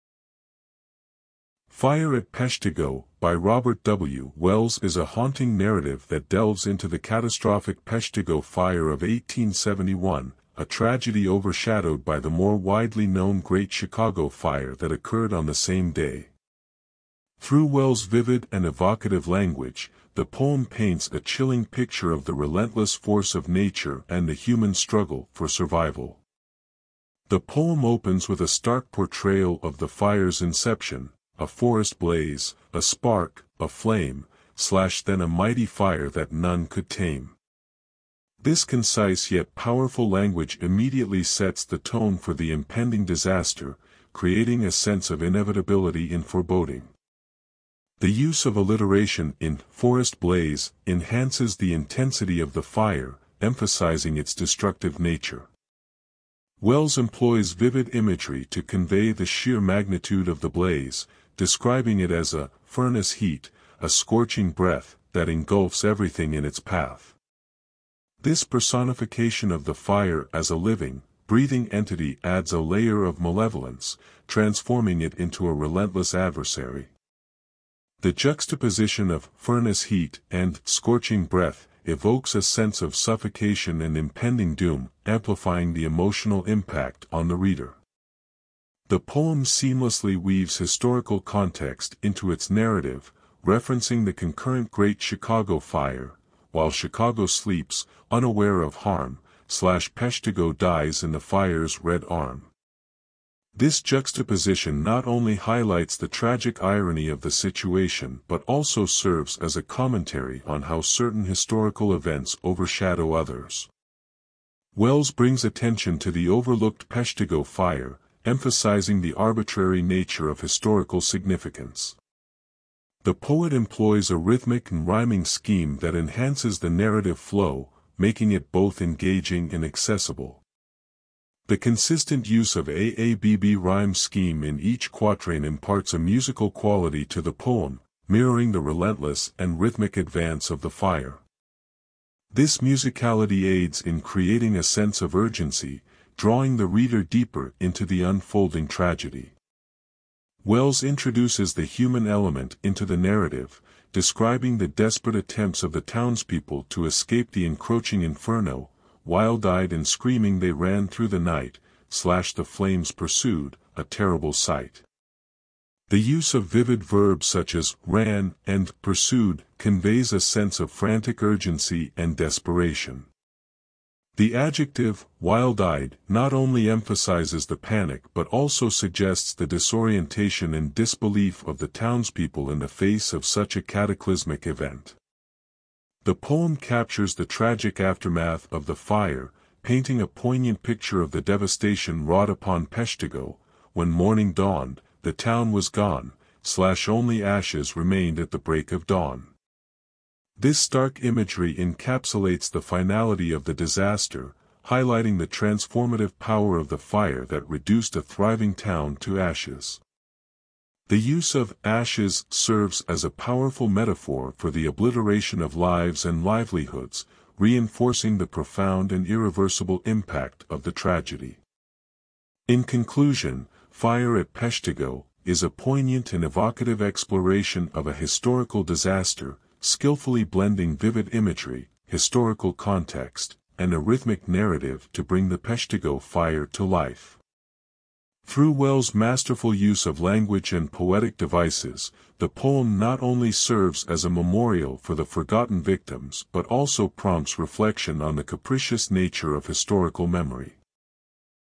"Fire at Peshtigo" By Robert W. Wells
"Fire at Peshtigo" by Robert W. Wells is a haunting narrative that delves into the catastrophic Peshtigo fire of 1871, a tragedy overshadowed by the more widely known Great Chicago Fire that occurred on the same day. Through Wells' vivid and evocative language, the poem paints a chilling picture of the relentless force of nature and the human struggle for survival.The poem opens with a stark portrayal of the fire's inception: "A forest blaze, a spark, a flame, / Then a mighty fire that none could tame." This concise yet powerful language immediately sets the tone for the impending disaster, creating a sense of inevitability and foreboding. The use of alliteration in "forest blaze" enhances the intensity of the fire, emphasizing its destructive nature.Wells employs vivid imagery to convey the sheer magnitude of the blaze, describing it as a "furnace heat, a scorching breath" that engulfs everything in its path. This personification of the fire as a living, breathing entity adds a layer of malevolence, transforming it into a relentless adversary. The juxtaposition of "furnace heat" and "scorching breath" evokes a sense of suffocation and impending doom, amplifying the emotional impact on the reader.The poem seamlessly weaves historical context into its narrative, referencing the concurrent Great Chicago Fire: "While Chicago sleeps, unaware of harm, / Peshtigo dies in the fire’s red arm." This juxtaposition not only highlights the tragic irony of the situation but also serves as a commentary on how certain historical events overshadow others. Wells brings attention to the overlooked Peshtigo fire, emphasizing the arbitrary nature of historical significance.The poet employs a rhythmic and rhyming scheme that enhances the narrative flow, making it both engaging and accessible. The consistent use of AABB rhyme scheme in each quatrain imparts a musical quality to the poem, mirroring the relentless and rhythmic advance of the fire. This musicality aids in creating a sense of urgency, drawing the reader deeper into the unfolding tragedy.Wells introduces the human element into the narrative, describing the desperate attempts of the townspeople to escape the encroaching inferno: "Wild-eyed and screaming they ran through the night, / The flames pursued, a terrible sight." The use of vivid verbs such as "ran" and "pursued" conveys a sense of frantic urgency and desperation. The adjective "wild-eyed" not only emphasizes the panic but also suggests the disorientation and disbelief of the townspeople in the face of such a cataclysmic event.The poem captures the tragic aftermath of the fire, painting a poignant picture of the devastation wrought upon Peshtigo: "When morning dawned, the town was gone, / Only ashes remained at the break of dawn." This stark imagery encapsulates the finality of the disaster, highlighting the transformative power of the fire that reduced a thriving town to ashes. The use of "ashes" serves as a powerful metaphor for the obliteration of lives and livelihoods, reinforcing the profound and irreversible impact of the tragedy.In conclusion, "Fire at Peshtigo" is a poignant and evocative exploration of a historical disaster, skillfully blending vivid imagery, historical context, and a rhythmic narrative to bring the Peshtigo fire to life. Through Wells' masterful use of language and poetic devices, the poem not only serves as a memorial for the forgotten victims but also prompts reflection on the capricious nature of historical memory.